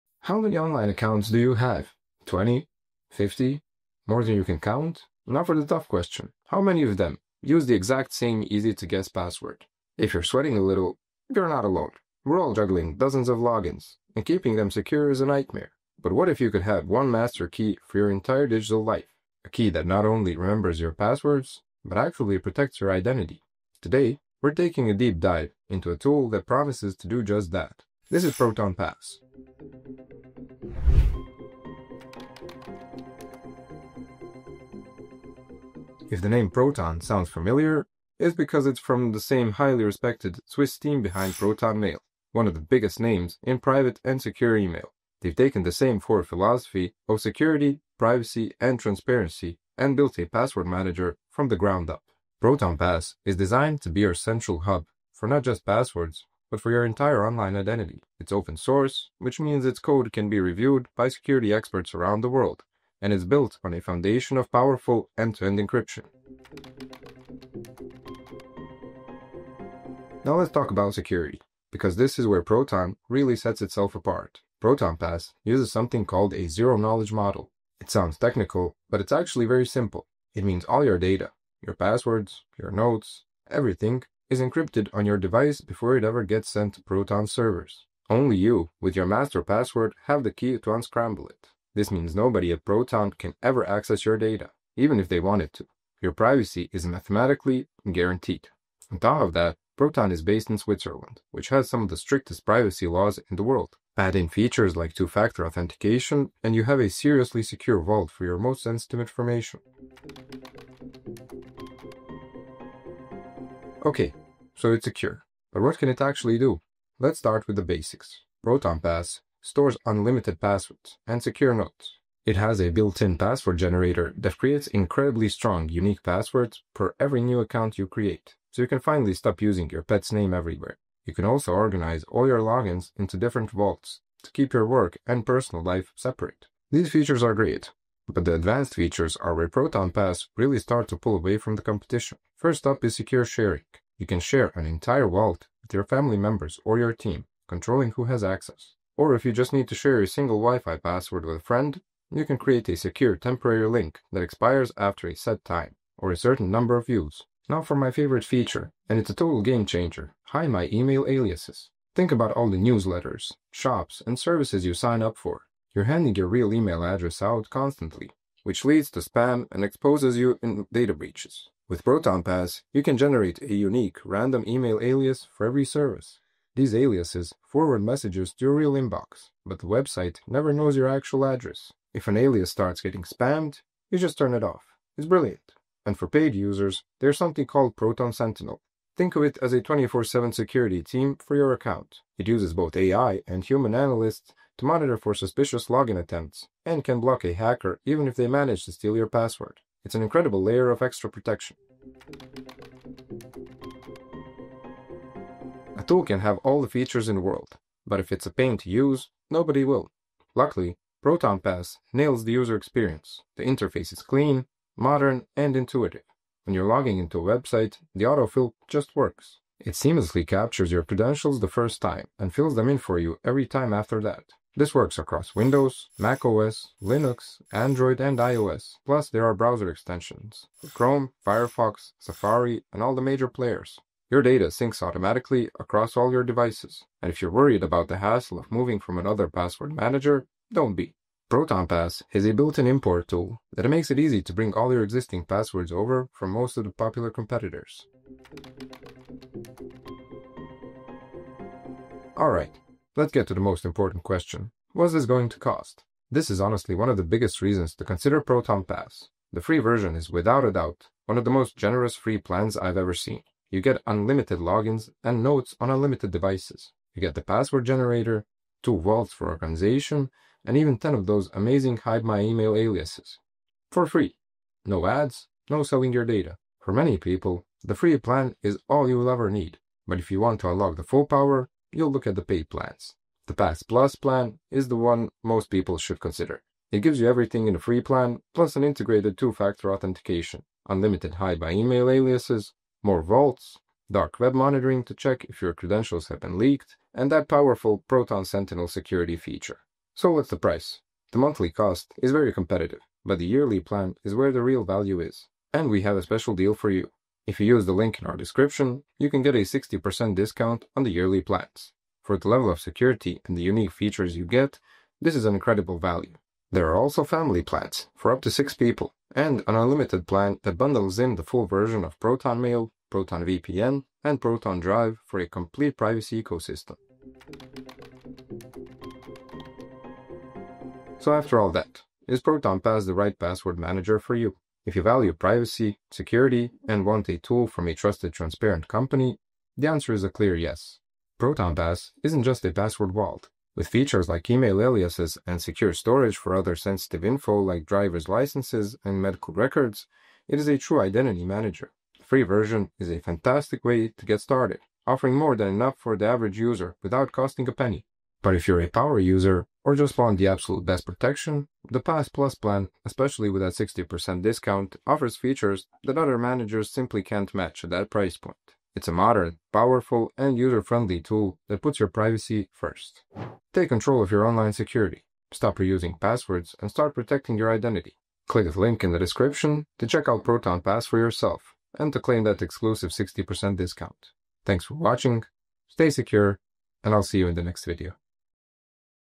Proton Pass Review - Is it the Best Password Manager?
Data brokers are quietly trading your personal info, your name, address, phone number, and more.
Unless you act, it stays on the internet... forever.

In this episode, we dive deep into Proton’s newest launch: Proton Pass.
It’s more than just a password manager, here’s how it stacks up against the competition:

-Syncing across devices
-Free vs premium features
-Autofill speed & accuracy
-Integration with Proton ecosystem
-Encryption & zero-access architecture
-Versus Bitwarden and 1Password

Chapters:
0:00 Intro
0:31 What is Proton Pass?
1:06 Top-Tier Security
1:49 Features That Actually Matter
3:21 User Experience & Platforms
4:08 The Price: Free vs. Paid
5:27 Final Verdict: Is It Right For You?

🔒 Real humans, real tests.
🧠 No bias, no clickbait.
📅 Weekly privacy & cybersecurity breakdowns.